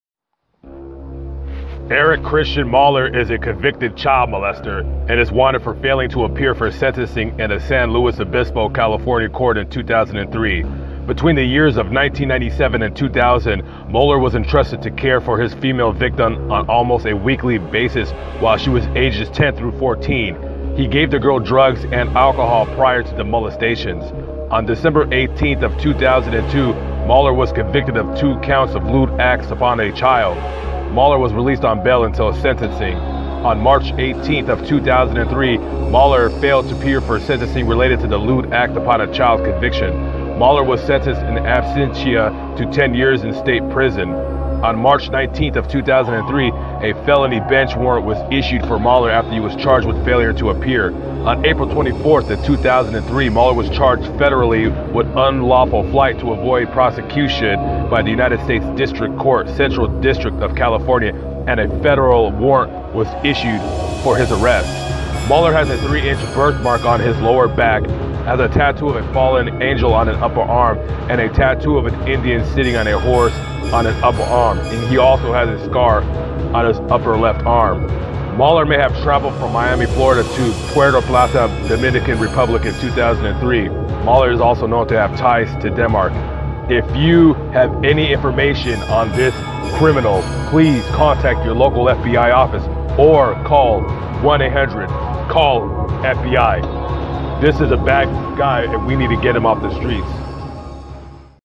Wanted for crimes against children---Erik Kristian Moller--
If you have any information on the whereabouts of this man please contact your local US Embassy or called the FBI at 1-800 call FBI you can also submit your tip anonymously at fbi.gov